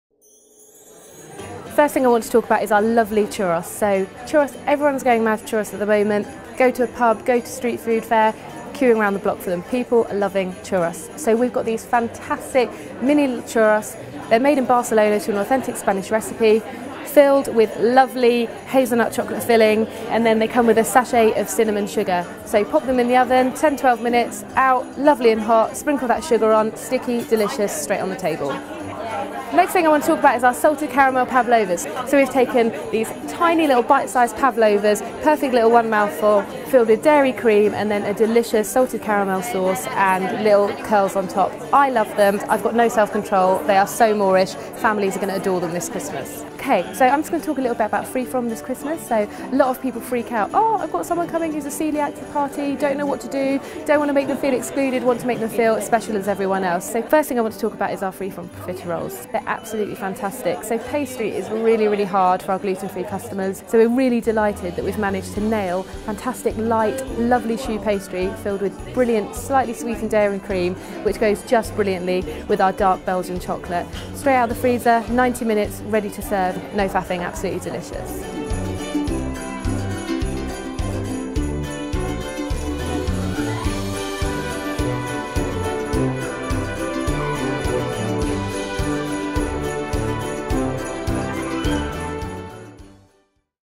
Tesco | Just Desserts
Product development experts Fiona Carter picks out her favourite desserts for the festive season including delicious churros.